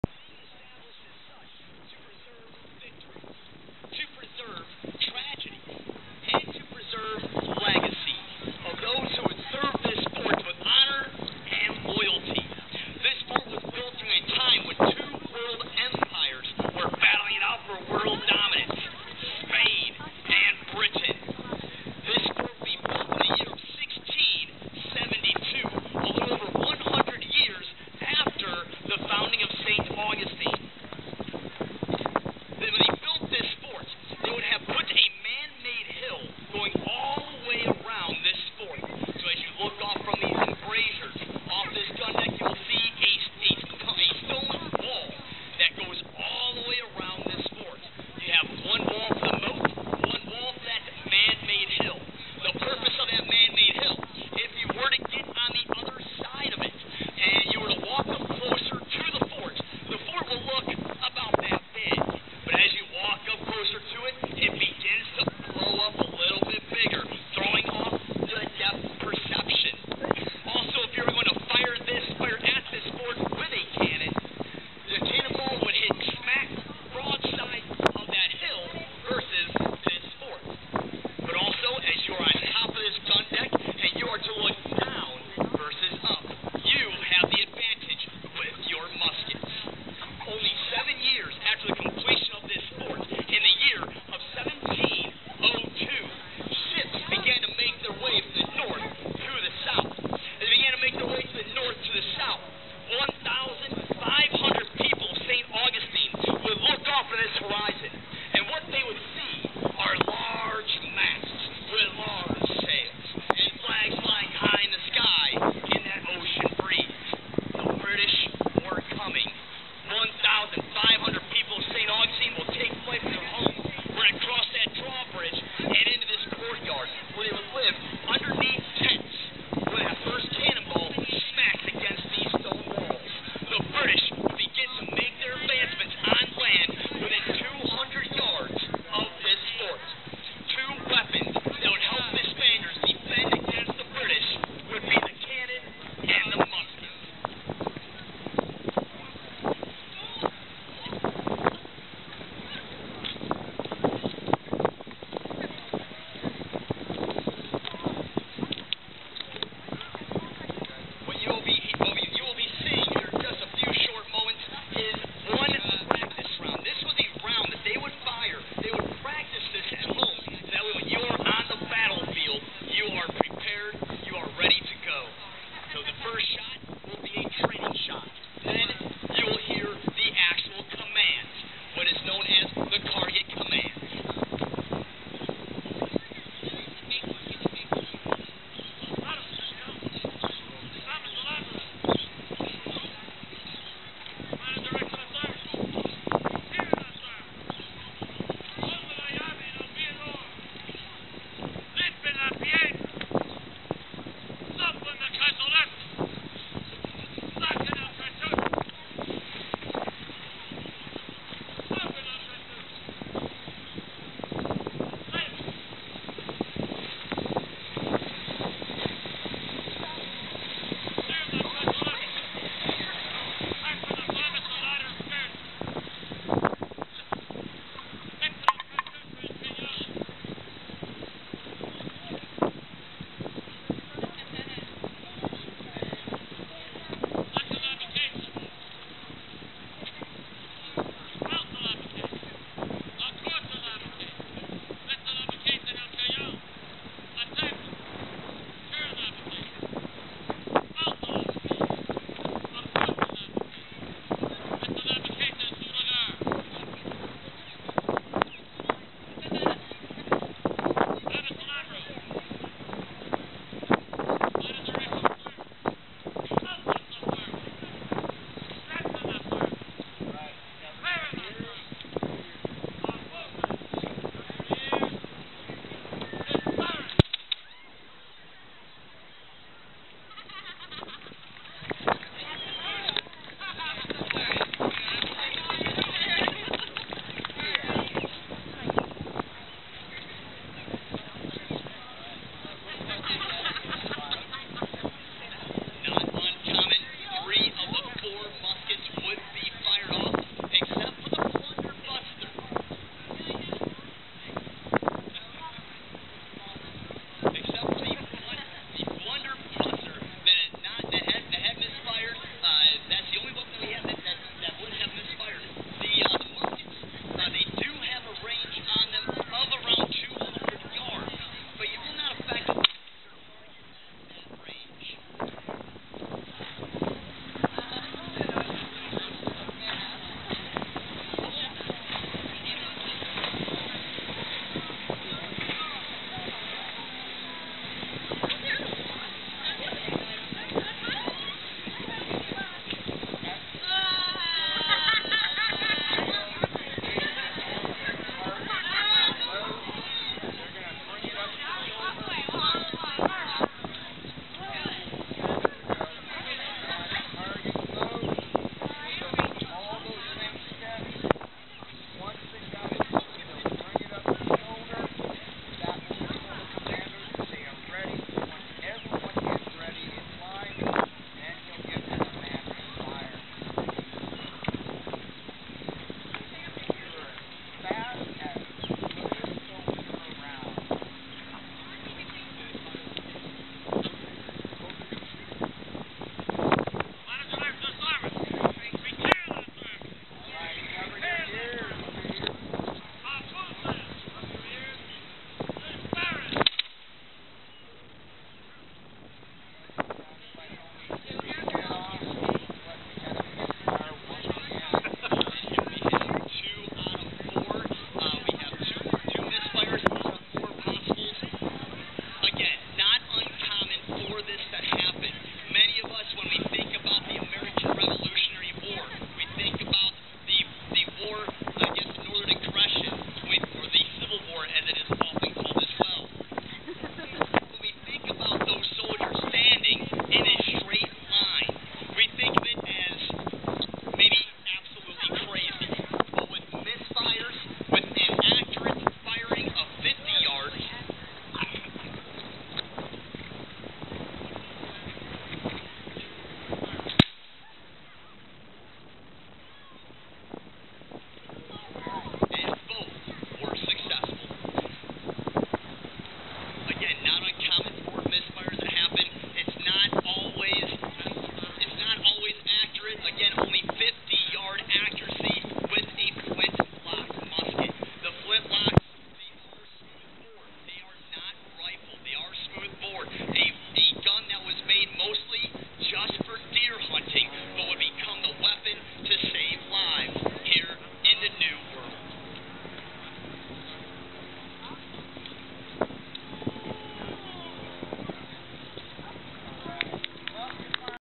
Musket Drill Demo at Castillo de San Marcos
History of Fort relating to demo of muskets.